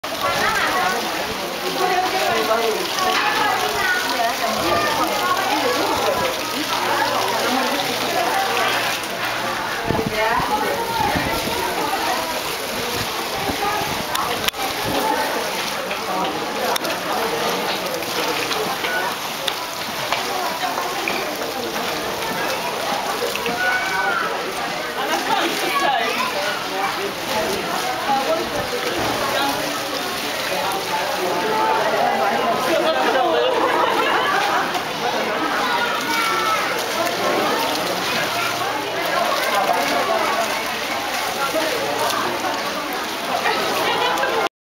15 May 2010 -1
Sea Lion playing with water